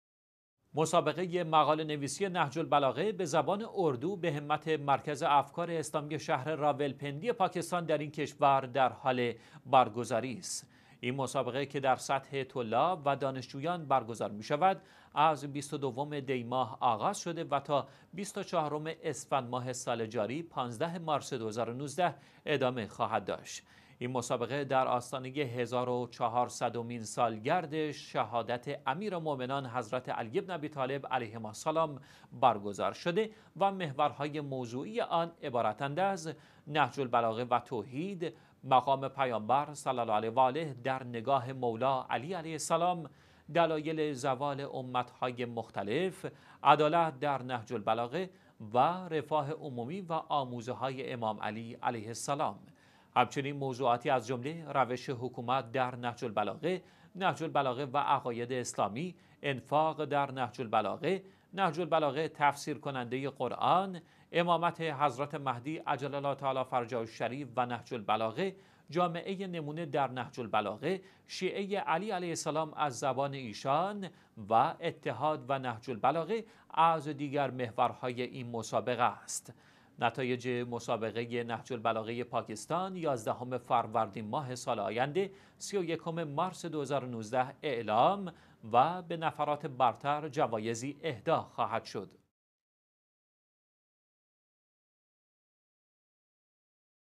برگزاری مسابقه مقاله نویسی نهج‌البلاغه در پاکستان
مسابقه «مقاله نویسی نهج البلاغه» به زبان اردو،  به همت «مرکز افکار اسلامی» شهر «راولپندی» پاکستان، در این کشور در حال برگزاری است.

این مسابقه که در سطح طلاب و دانشجویان برگزار می‌شود، از  ۲۲ دی ماه آغاز شده و تا ۲۴ اسفندماه سال جاری (۱۵ مارس ۲۰۱۹) ادامه خواهد داشت.

این مسابقه در آستانه هزار و چهارصدمین سالگرد شهادت امیرالمومنین حضرت علی بن ابی طالب (علیهما سلام) برگزار شده، و محورهای موضوعی آن عبارتند از:  «نهج البلاغه و توحید»، «مقام پیامبر صلی الله علیه و آله در نگاه مولا علی علیه السلام»،  «دلایل زوال امت‌های مختلف»،  «عدالت در نهج البلاغه»،  و «رفاه عمومی و آموزه‌های امام علی علیه السلام».

همچنین موضوعاتی از جمله: «روش حکومت در نهج البلاغه»، «نهج البلاغه و عقاید اسلامی»، «انفاق در نهج البلاغه»، «نهج البلاغه؛ تفسیرکننده قرآن»، «امامت حضرت مهدی عجل الله فرجه و نهج البلاغه»، «جامعه نمونه در نهج البلاغه»، «شیعه علی علیه السلام از زبان ایشان» و «اتحاد و نهج البلاغه» از دیگر محورهای این مسابقه است.

نتایج مسابقه نهج البلاغه پاکستان،  ۱۱ فروردین ماه سال آینده (۳۱ مارس ۲۰۱۹)، اعلام و به نفرات برتر جوایزی اهدا خواهد شد.